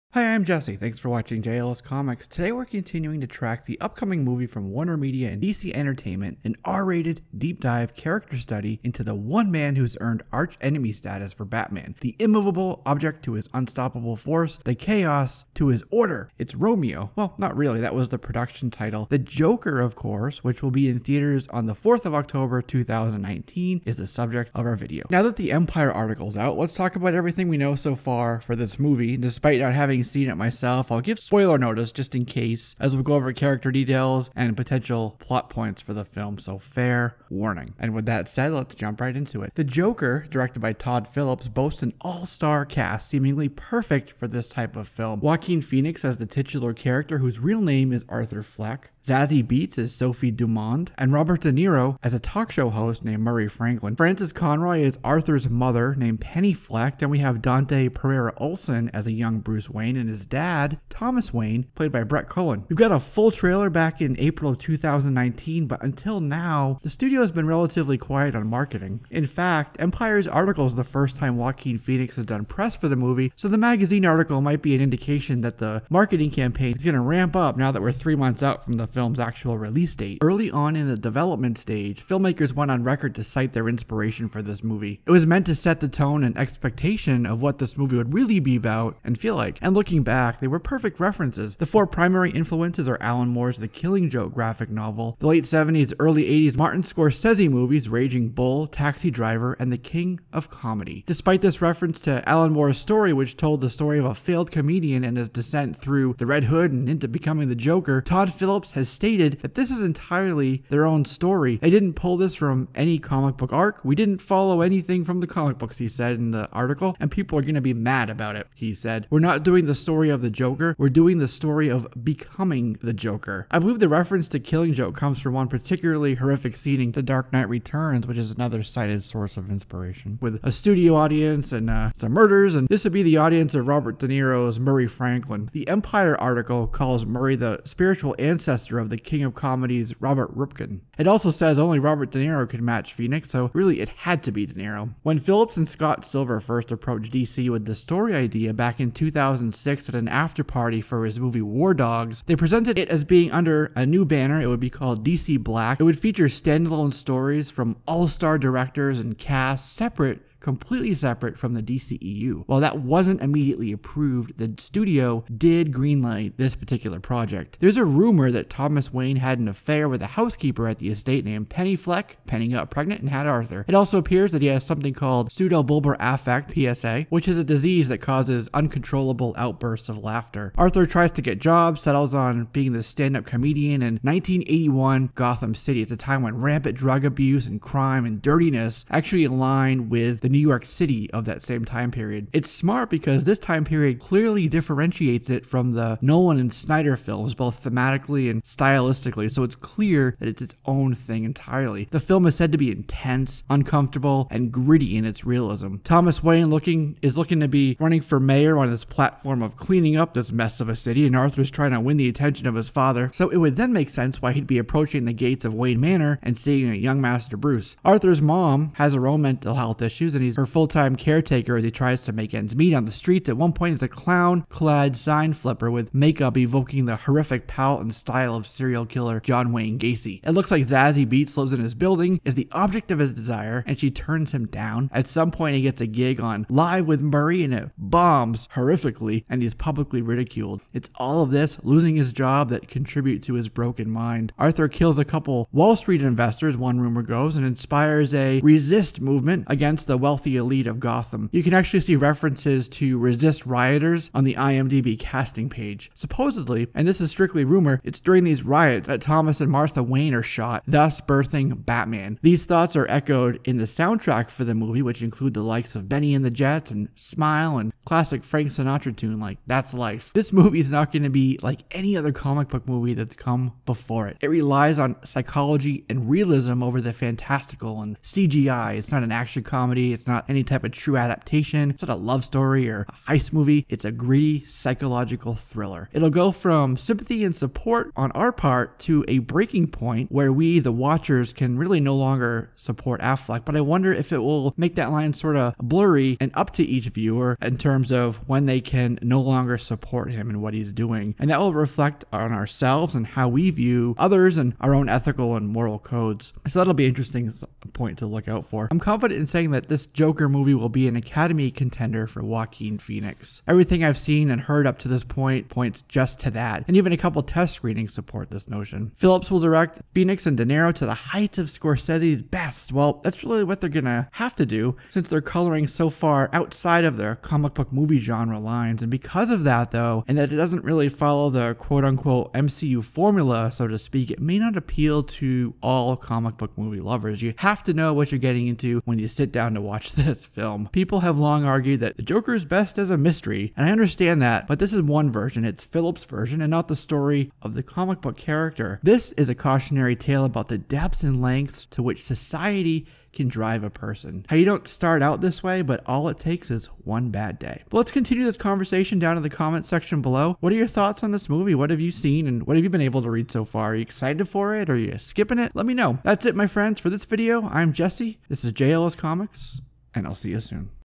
DC's The JOKER Movie: Everything We Know So Far! Empire Magazine Reveals
Now that Empire Magazine's article on Todd Phillips' 'The Joker' movie is out, we know a lot more about the Joaquin Phoenix helmed project ... here's everything we know so far!

WARNING: Massive potential spoilers in the video!